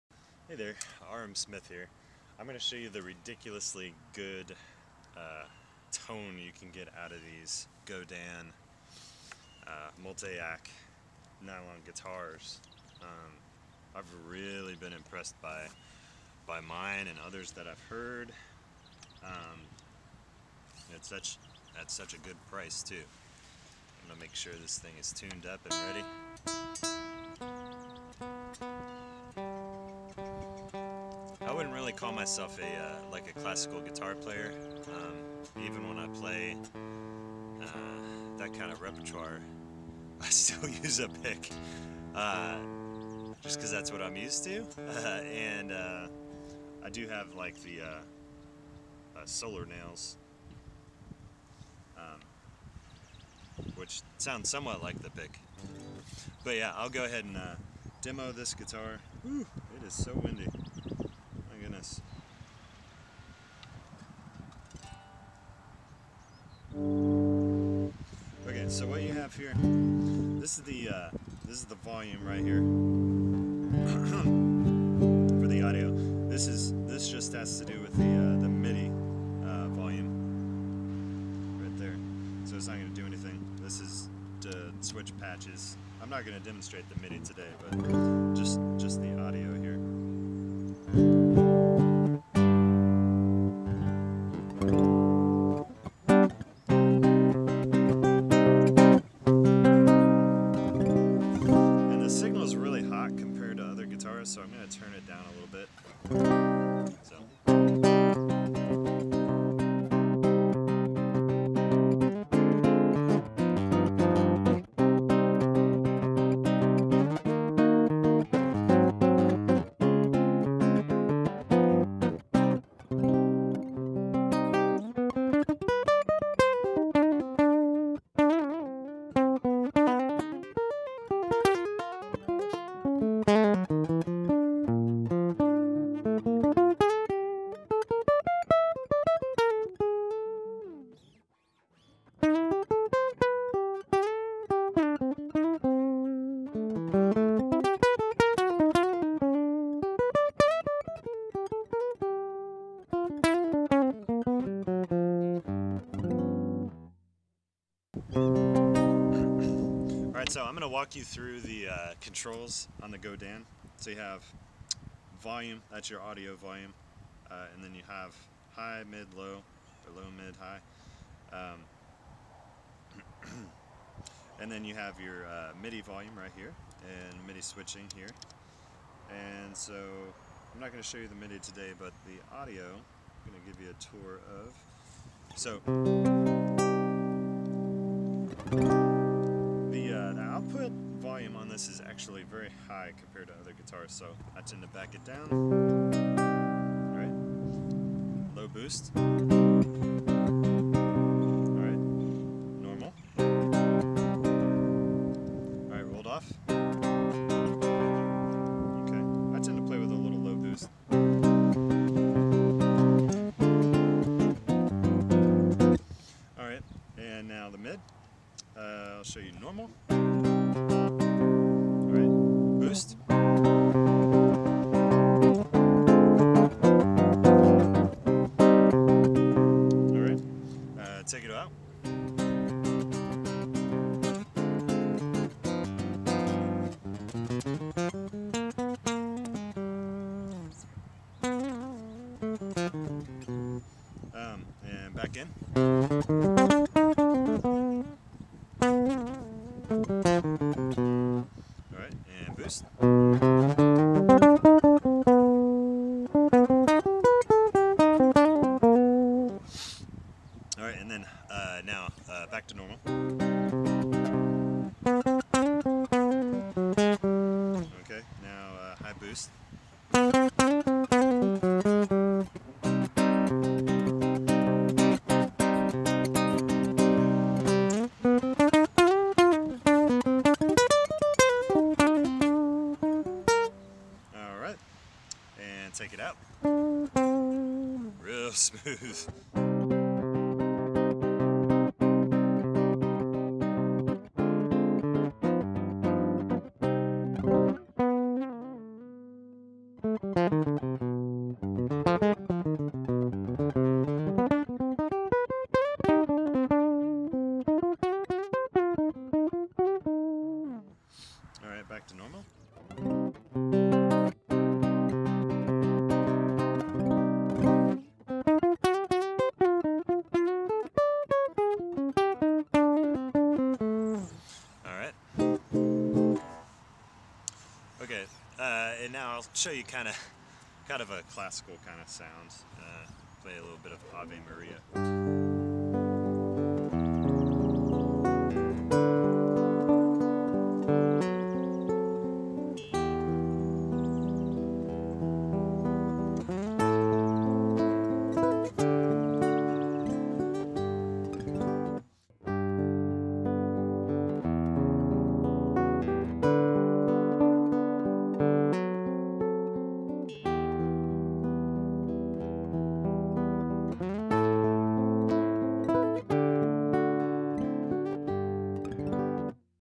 RM Smith Godin ACS SA Rosewood Demo
In this video, I demonstrate my Godin ACS SA Rosewood classical nylon guitar.  I will admit the sound of the guitar through a P.A. or acoustic amp (I use a Fishman Loudbox) is much better than the straight DI sound.  However, you get to hear a completely unprocessed sound coming straight from the guitar.  Enjoy!